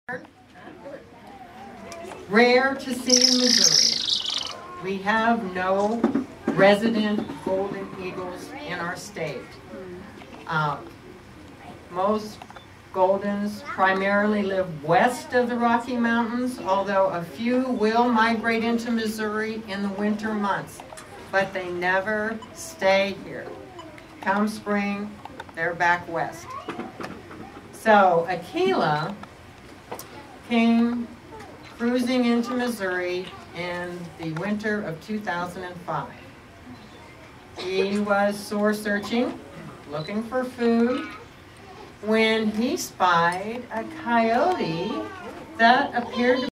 Aquila the golden eagle
Aquila the golden eagle from Dickerson Park Zoo in Springfield Mo. Video taken at Eagle Days at the Nature Center